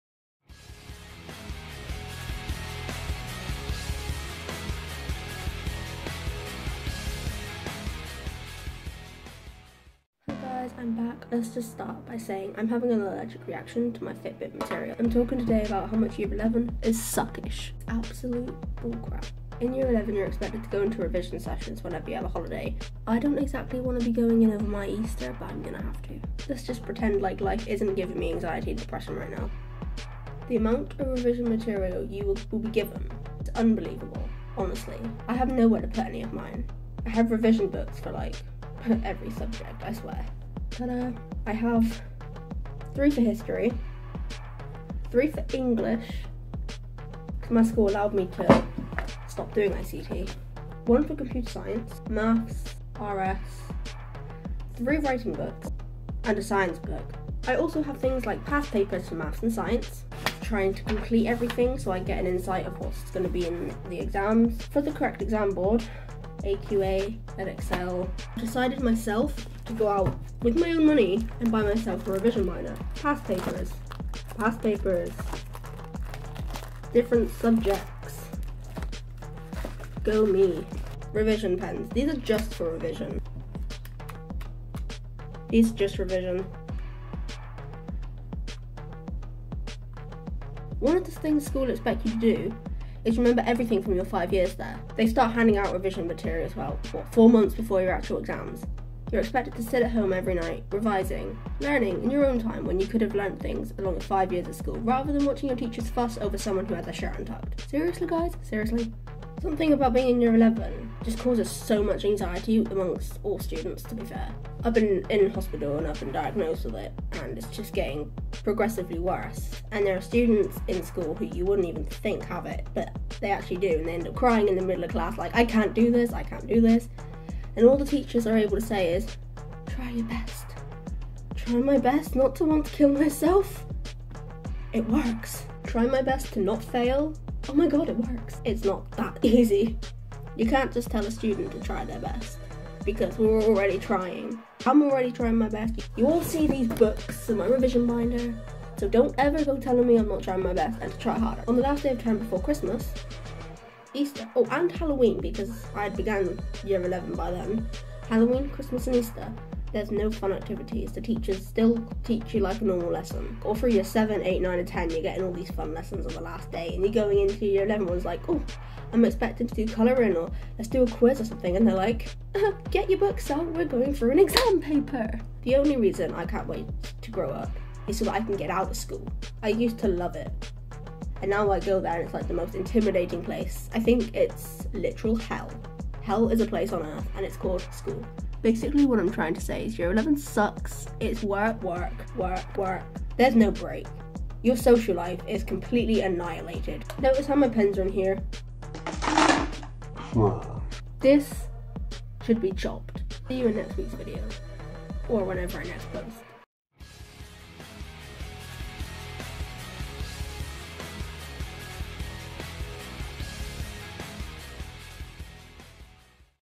Year 11... SUCKS!
Hey hey i have created a new intro and outro! WHOOP!!
Hope you all like this video. Sorry i dont post as much as i should, its just difficult because i constantly have my siblings around making the place less quiet haha.